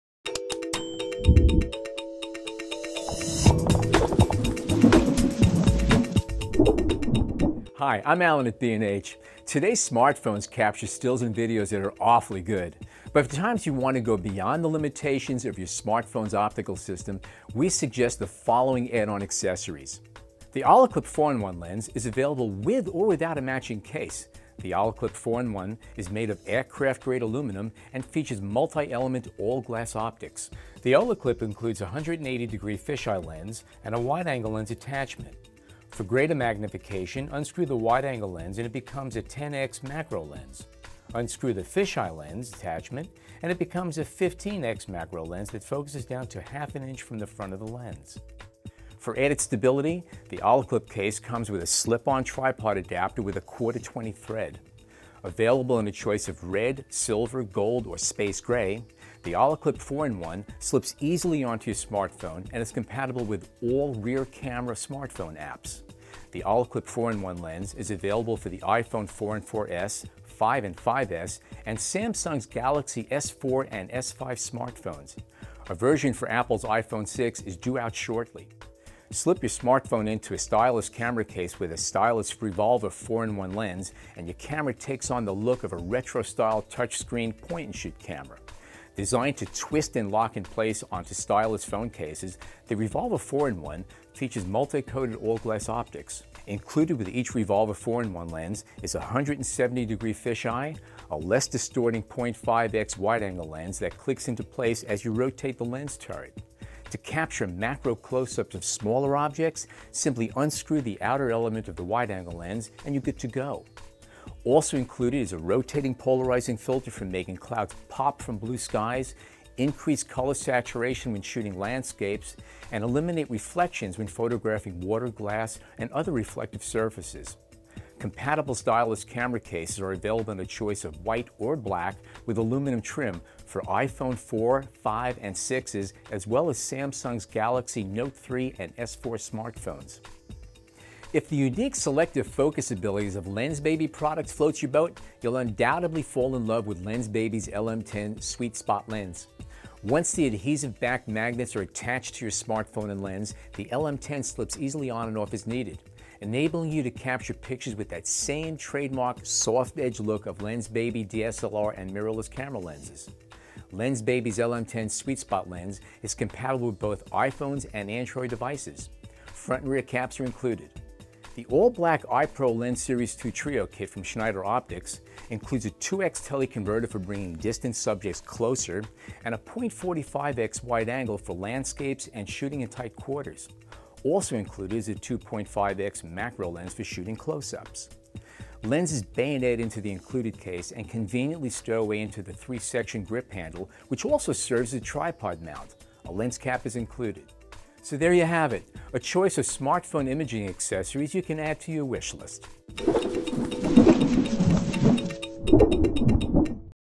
Wish List: Smartphone Lens Attachments
Shop at B&H:
In this Wish List video, B&H goes through some interesting options for smartphone lens attachments that can enhance your smartphone and iPhone photography.